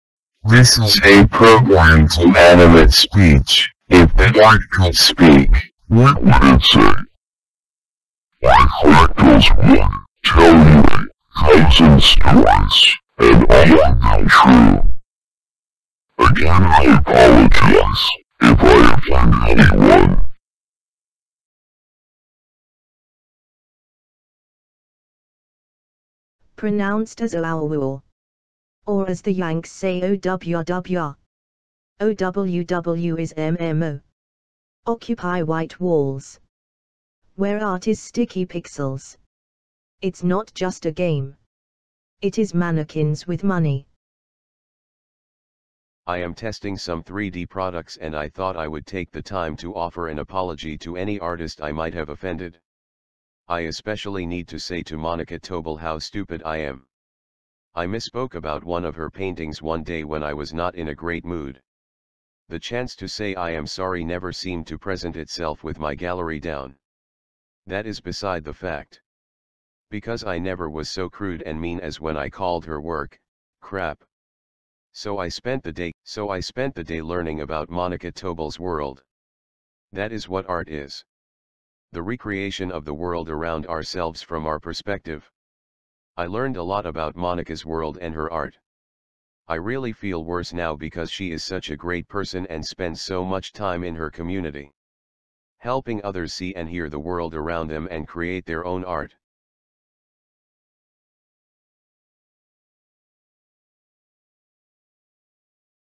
OWW MMO.
TESTING ANIMATED SPEECH.
Really simple and has more options than most animated programs for $50.
Imagine having an art Gallery where the paintings could tell you about themselves.